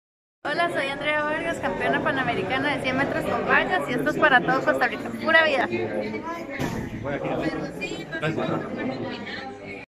Mensaje de Andrea Vargas, campeona en Lima 2019
Después de su histórico triunfo en los 100 metros con vallas, Andrea Carolina Vargas envió este emotivo mensaje al Comité Olímpico Nacional, en el cual le hizo una dedicatoria especial al pueblo de Costa Rica, en su calidad de campeona panamericana en Lima 2019.